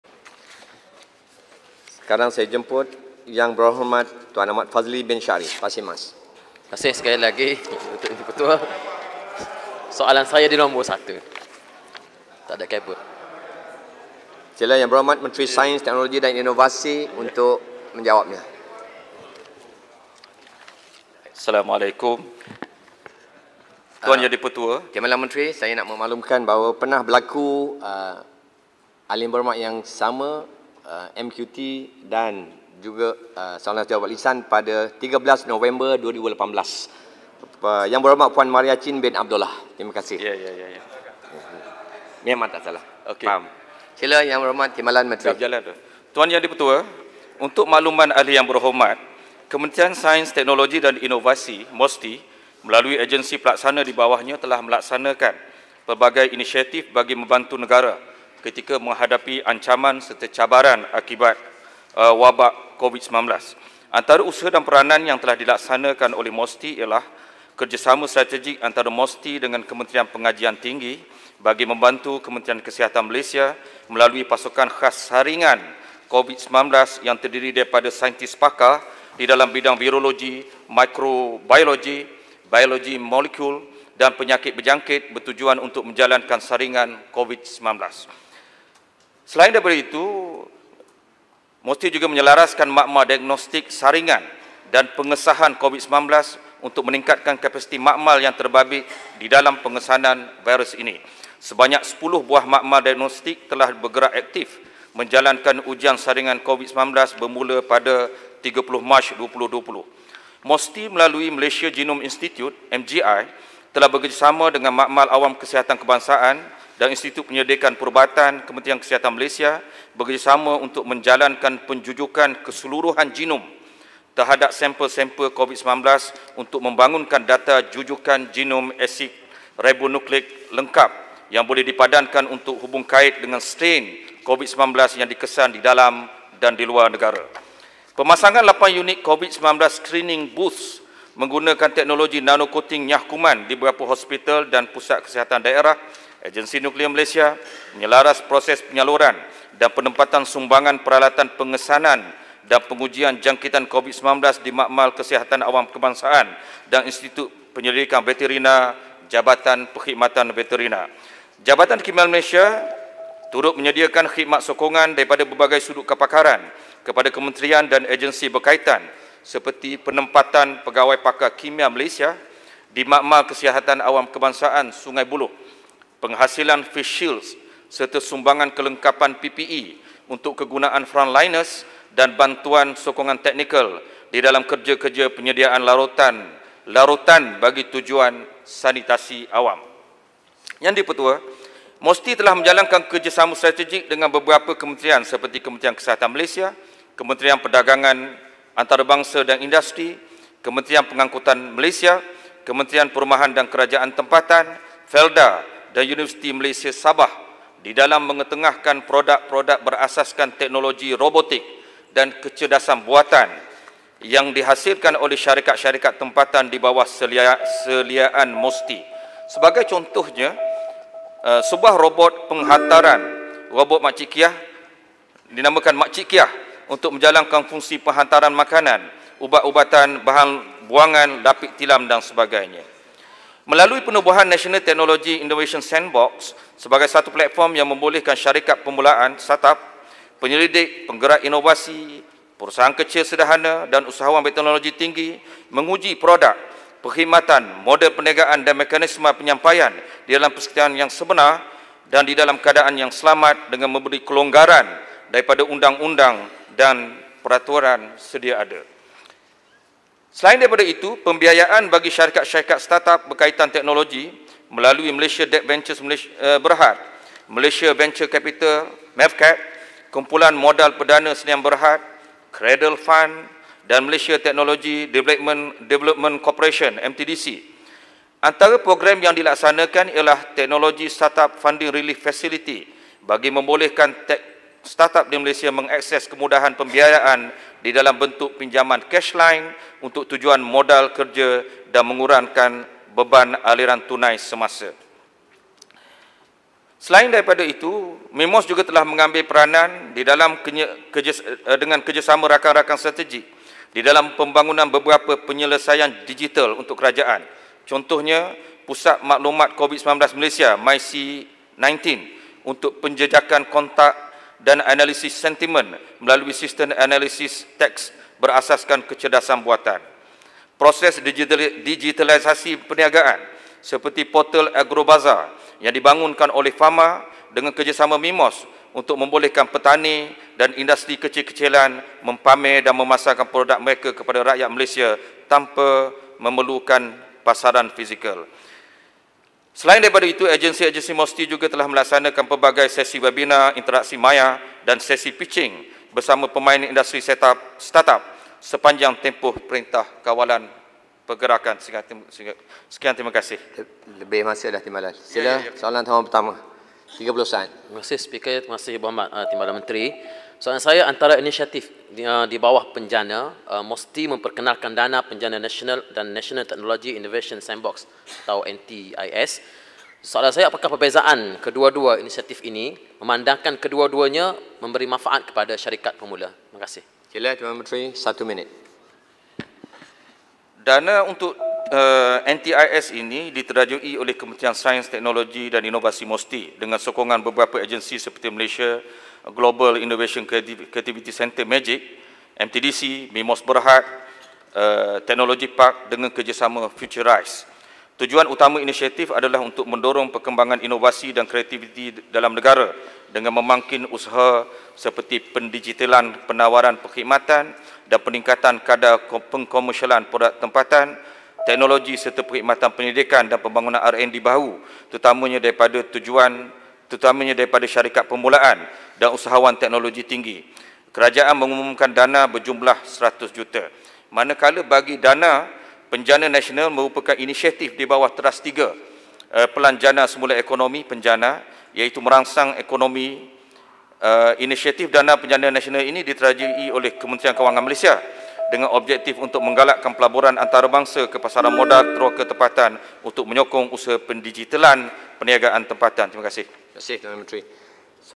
Peranan MOSTI Ketika Wabak Covid-19: YB Pasir Mas Minta Penjelasan
4 Ogos 2020 | Peranan Mosti Hadapi Covid-19

Soalan Ahli Parlimen Pasir Mas, YB Ustaz Ahmad Fadhli Shaari